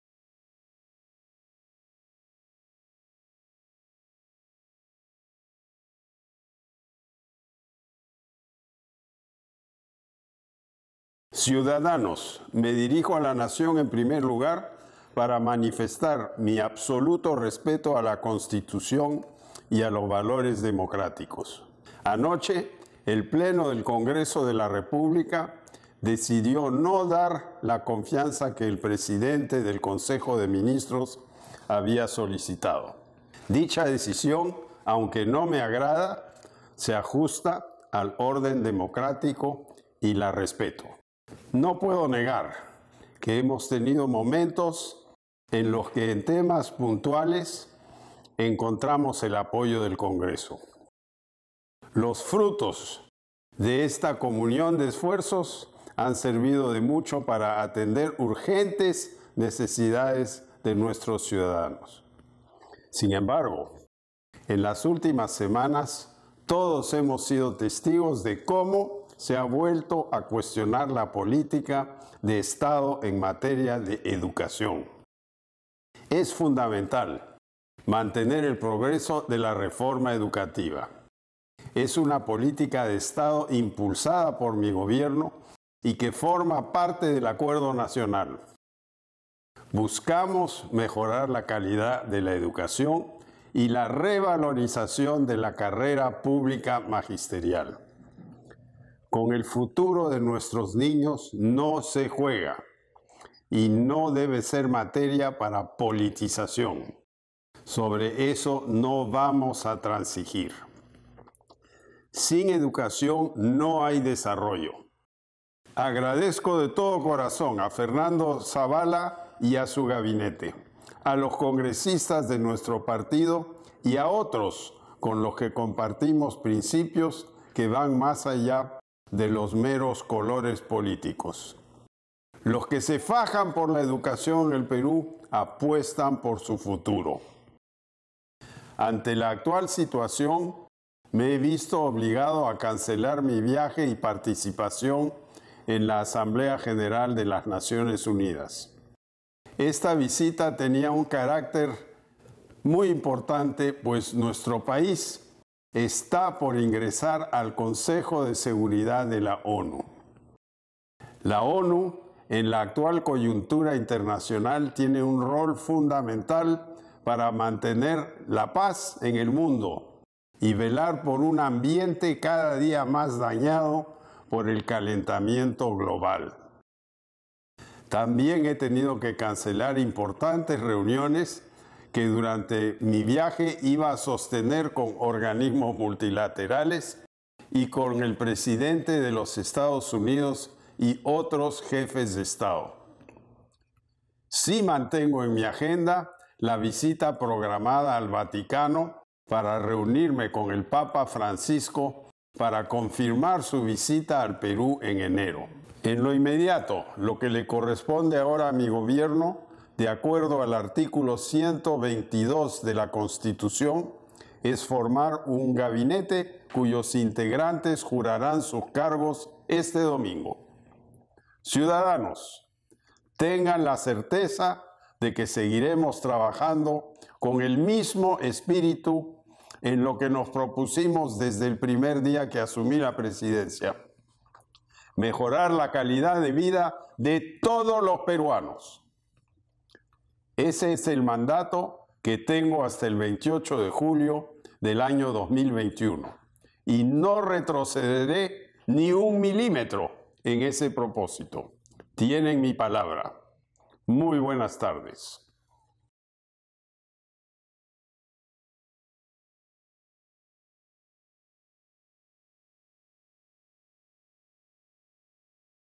Mensaje a la Nación
El presidente de la República, Pedro Pablo Kuczynski, anunció que el domingo 17 de setiembre juramentará el nuevo primer ministro y su Gabinete, y ratificó su absoluto respeto a la Constitución y a los valores democráticos. Aseguró que no permitirá que se ponga en peligro la reforma educativa, ni se juegue políticamente con el futuro de los niños.